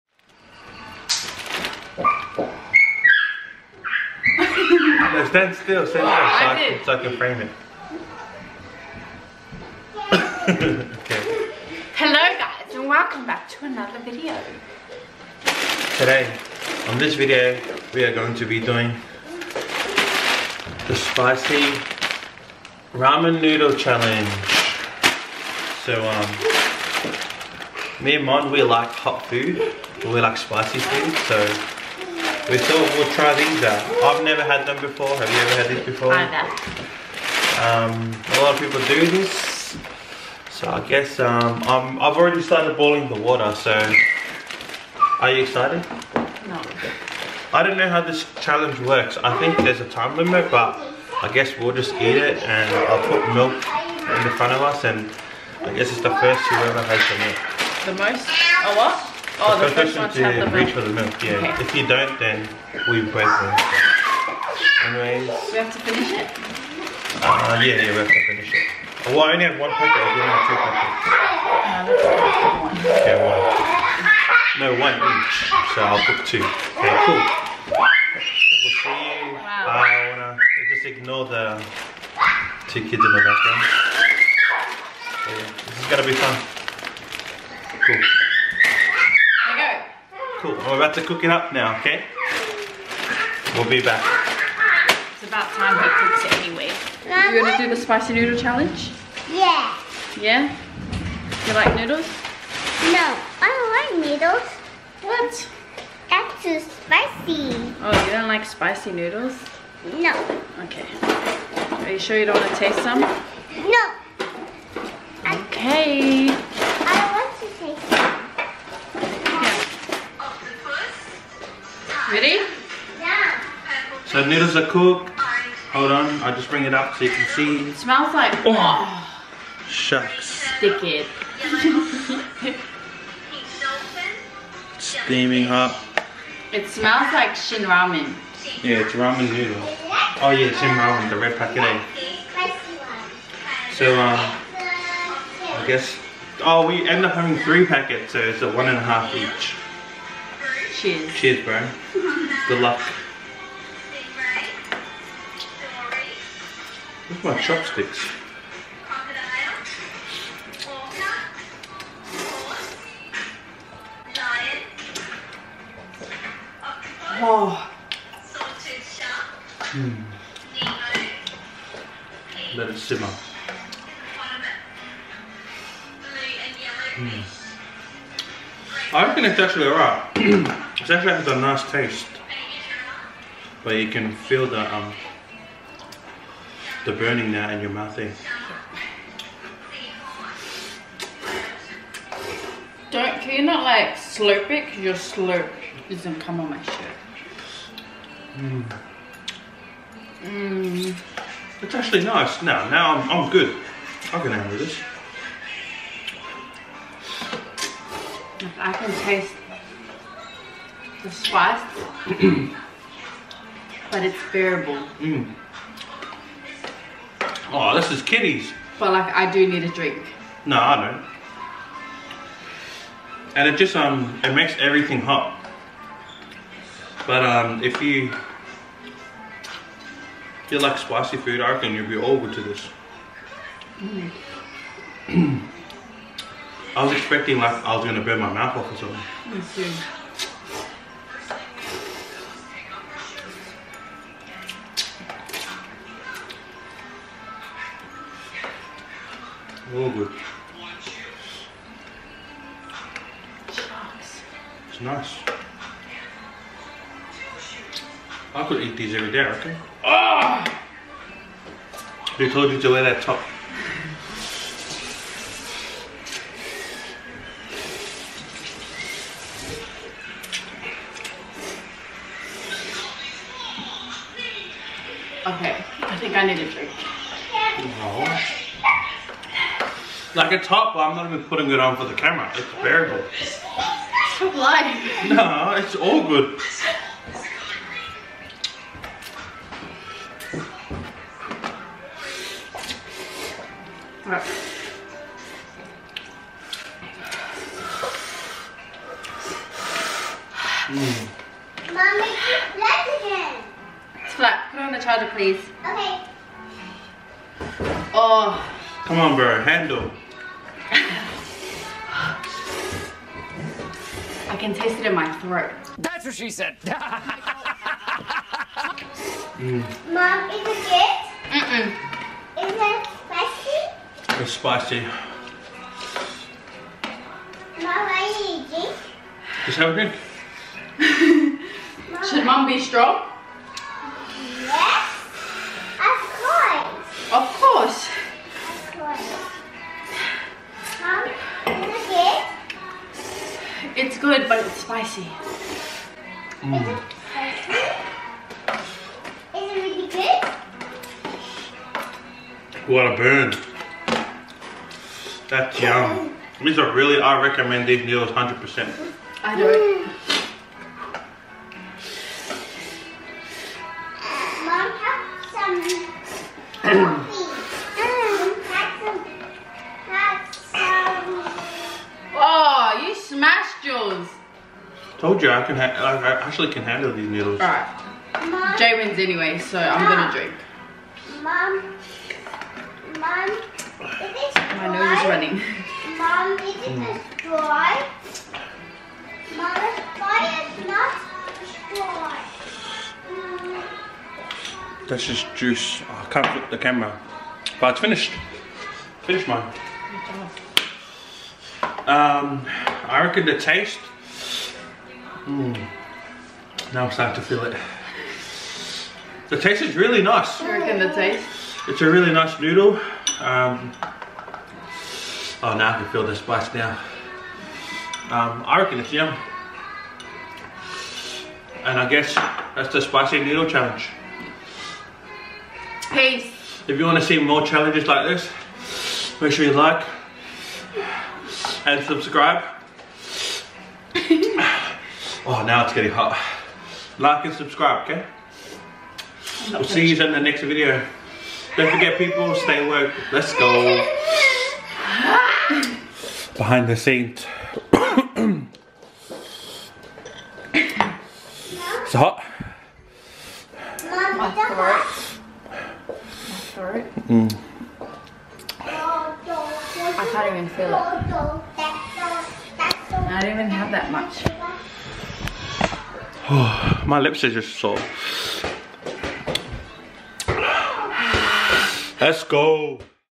SPICY RAMEN NOODLE CHALLENGE X2 IS KIDDIES !
Welcome to another Challenge Video

In this episode we try the Korean Samyang Spicy Ramen Noodles x2 Challenge.

The first to reach for the milk or water loses.

Who do you think caved ?

FOLLOW ME ON ALL SOCIALS FOR BEHIND THE SCENES CONTENT